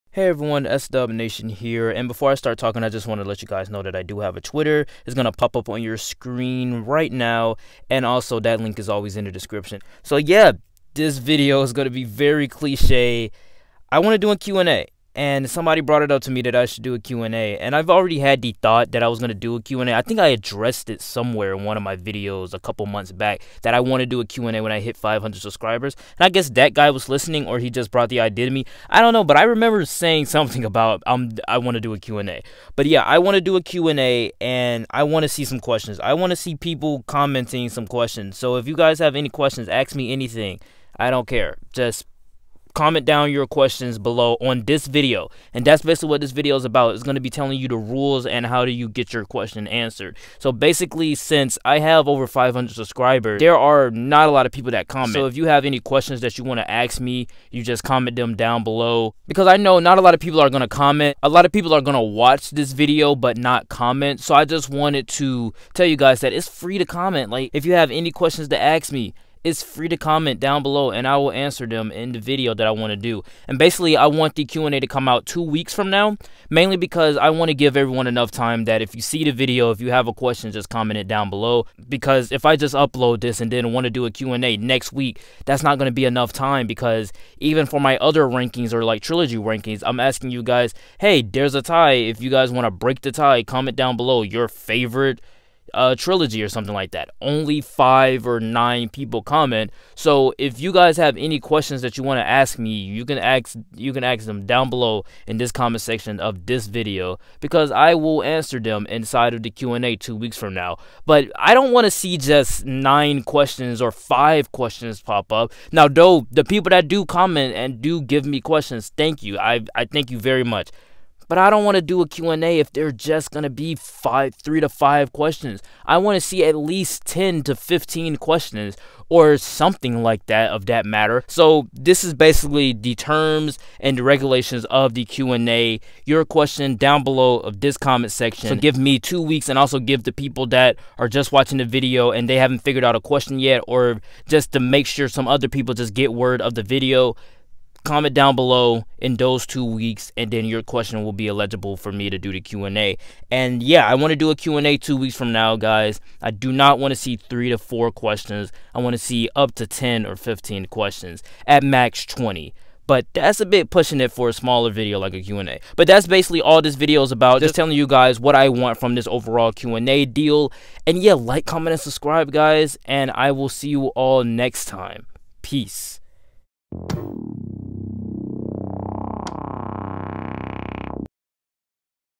Q AND A in 2 Weeks! [COMMENT YOUR QUESTIONS]
This video is about the details on how the Q and A will go!

* Q AND A IN TWO WEEKS COMMENT YOUR QUESTIONS*